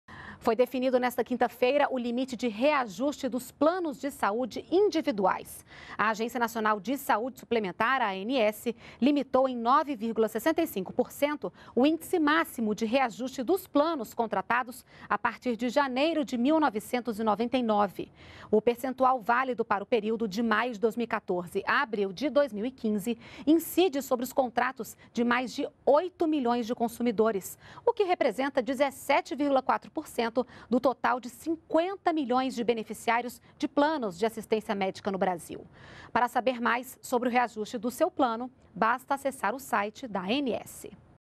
Reajuste nos valores de planos de saúde não pode ultrapassar 9,65%
NBR NOTÍCIAS - 03.07.14: A Agência Nacional de Saúde Suplementar (ANS) limitou em 9,65% o índice máximo de reajuste dos planos de saúde contratados a partir de janeiro de 1999. O percentual, válido para o período de maio de 2014 a abril de 2015, incide sobre os contratos de mais de 8 milhões de consumidores, o que representa 17,4% do total de 50 milhões de beneficiários de planos de assistência médica no Brasil.